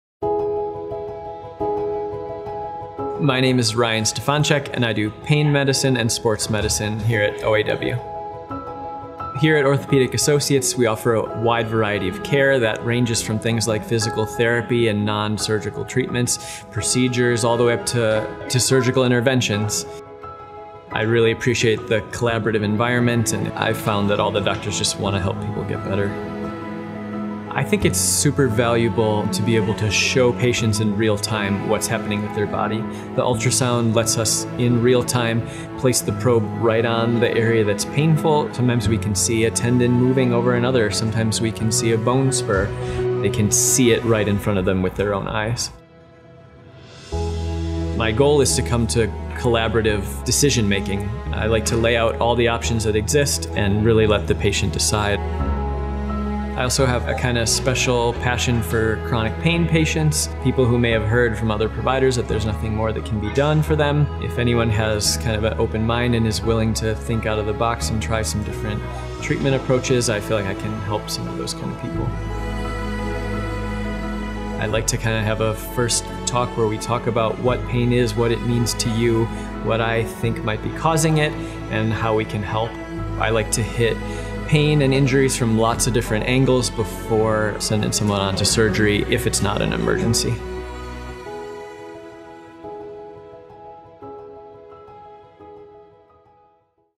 Ryan Stefanczyk, M.D. // Pain Management and Sports Medicine // Orthopaedic Associates of Wisconsin
Meet Dr. Ryan Stefancyzk, a dedicated pain and sports medicine specialist at Orthopedic Associates of Wisconsin (OAW), who is transforming how we approach pain management and sports injuries. In this video, Dr. Stefancyzk introduces the comprehensive range of treatments available at OAW, from physical therapy and non-surgical procedures to surgical interventions. He emphasizes the collaborative environment at OAW, where all doctors are committed to improving patient outcomes.

Join Dr. Stefancyzk as he shares his approach to understanding and treating pain, aiming to empower patients with knowledge and care options that can drastically improve their quality of life.